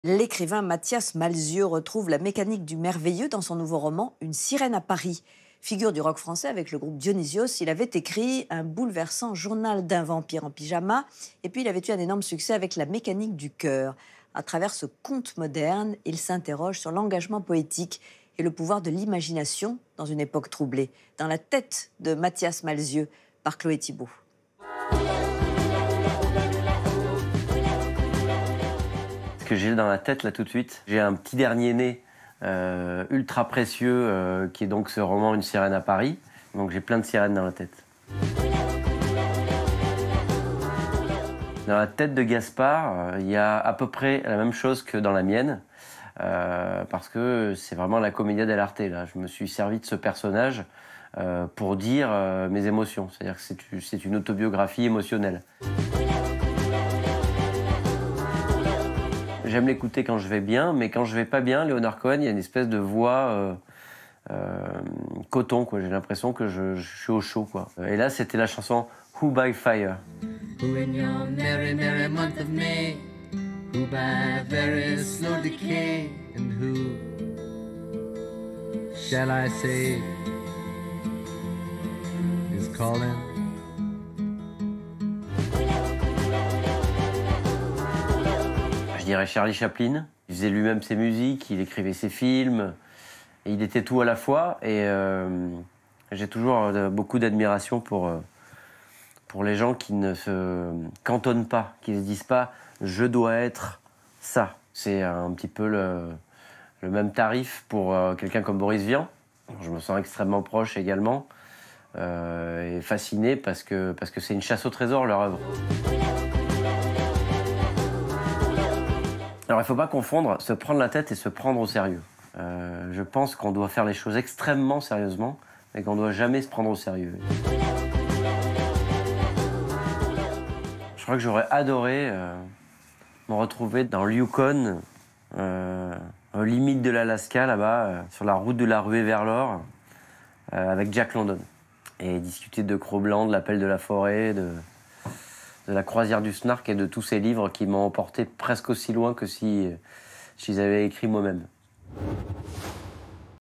Dans la tête de Mathias Malzieu
Le musicien et écrivain Mathias Malzieu retrouve la mécanique du merveilleux dans son nouveau roman « Une sirène à Paris ». Le membre du groupe rock Dionysos y raconte l’histoire d’amour impossible entre un homme et une sirène dans le Paris contemporain. Dans la tête de Mathias Malzieu.